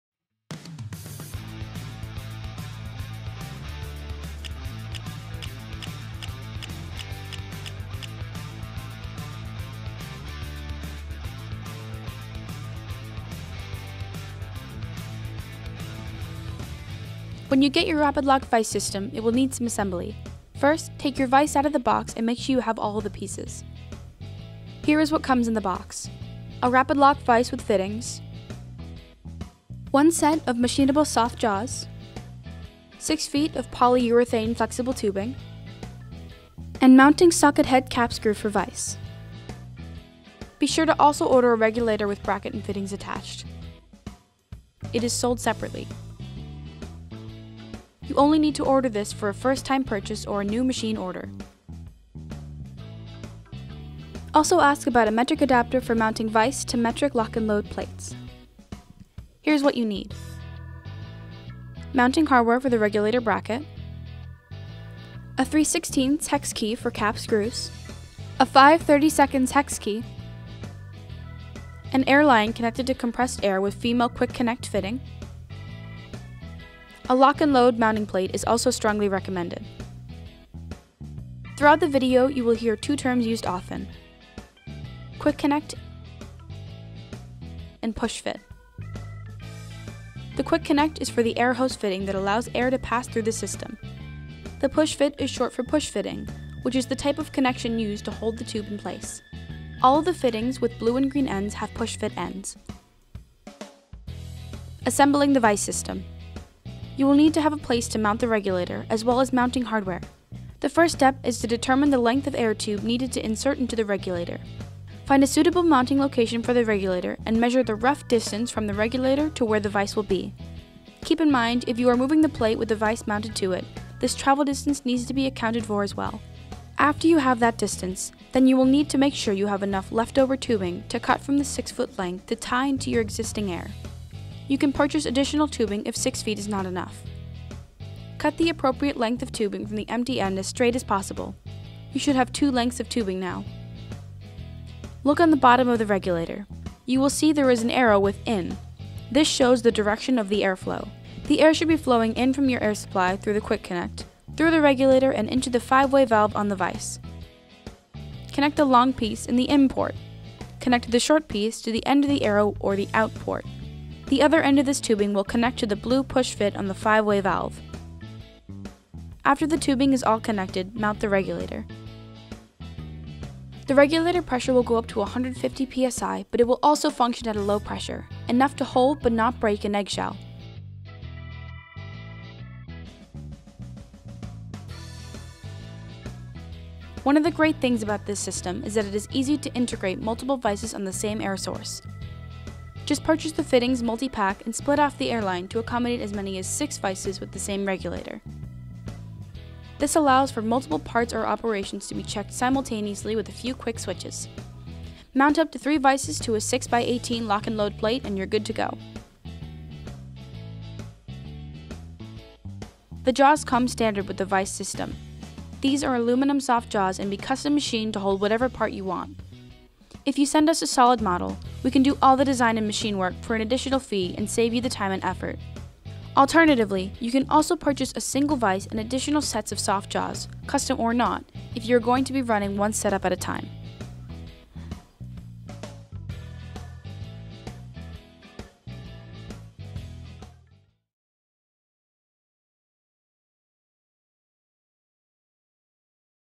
Rapid Loc™ Pneumatic Vise for CMM Inspection - the only like it on the market!
Rapid-Loc™ pneumatic vise for CMM Inspection is designed to take advantage of the air at your CMM. Soft jaws for the Rapid-Loc™ vise can be customized to hold most any part. Regulated air pressure can produce just enough clamping force to hold delicate parts or as much as 150-lbs if needed. Mount Rapid-Loc™ vises on Loc-N-Load™ quick-swap fixture plates for fast, accurate, and highly repeatable setups that help break inspection bottlenecks. Rapid-Loc™ can also be mounted on existing CMM breadboards.